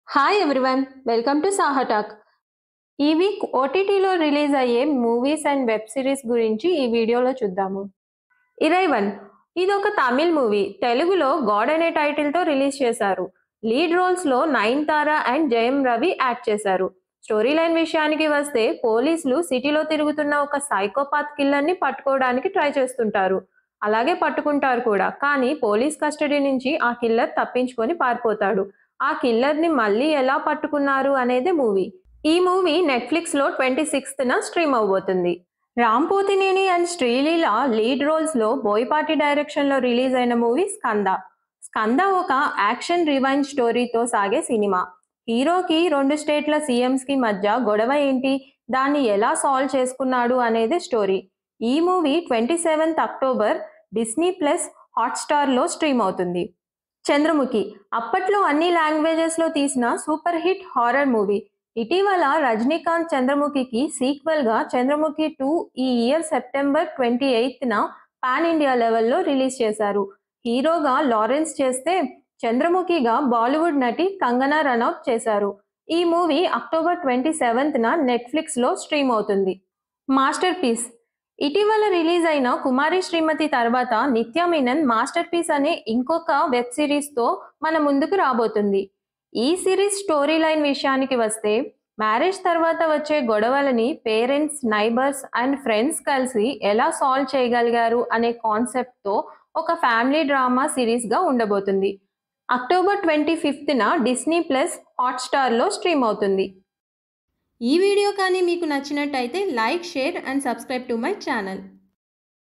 Latest movie and web series OTT release dates(OCT 23-29) | Upcoming Telugu OTT Releases
This video contains the latest movies and web series OTT release dates and streaming platform details. All the details provided in the video are gathered from different sources and dates might differ slightly.